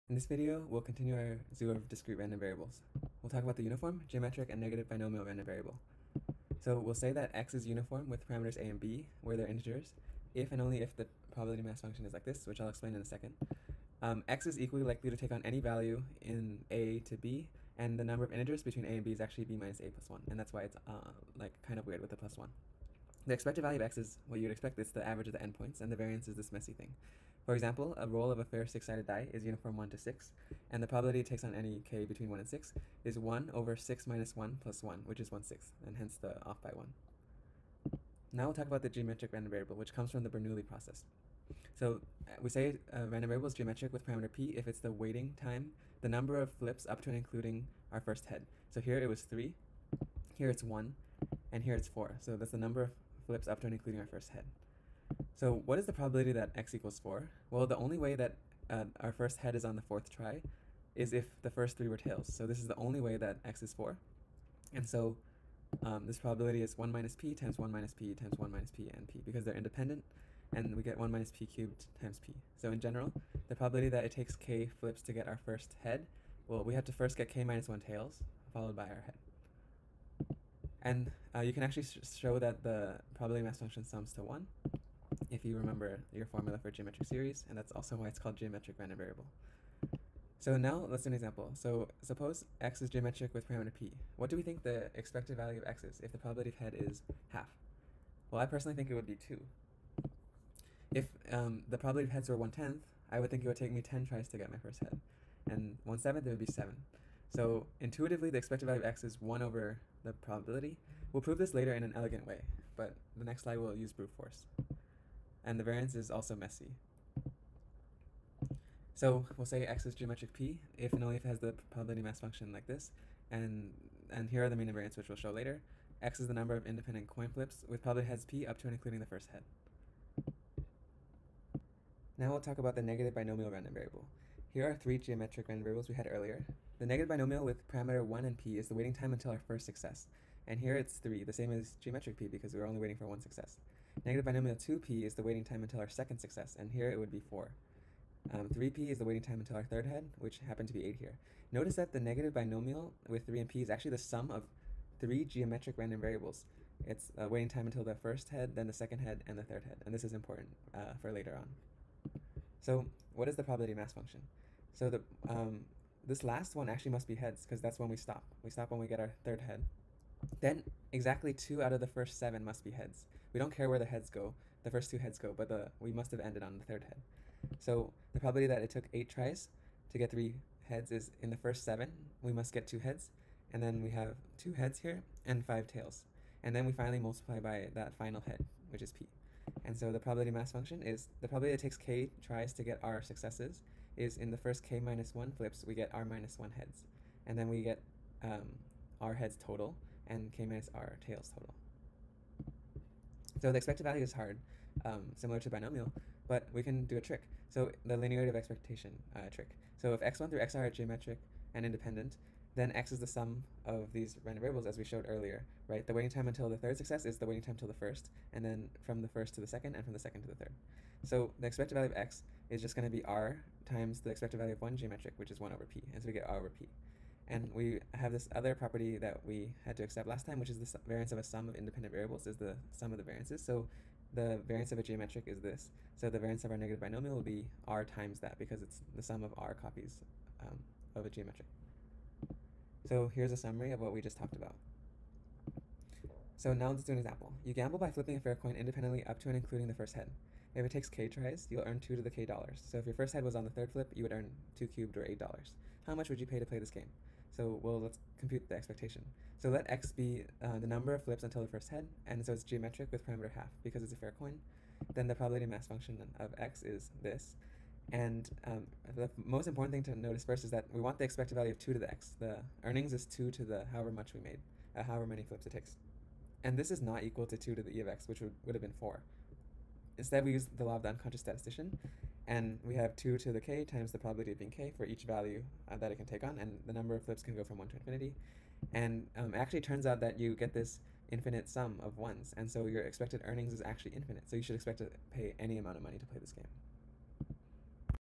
[3. Discrete RVs] 3.5 Zoo of Discrete RVs Part II
This series [Probability] closely follows Stanford University's CS 109 (Probability for Computer Scientists), and University of Washington's CSE 312 (Foundations of Computing II) lecture schedule.

The expected prerequisites are college calculus (including some multivariable calculus such as gradients and multiple integrals), and some introduction to proofs and discrete math.

This 5-minute video covers the following topics:
1. The (Discrete) Uniform Random Variable
2. The Geometric Random Variable
3. The Negative Binomial Random Variable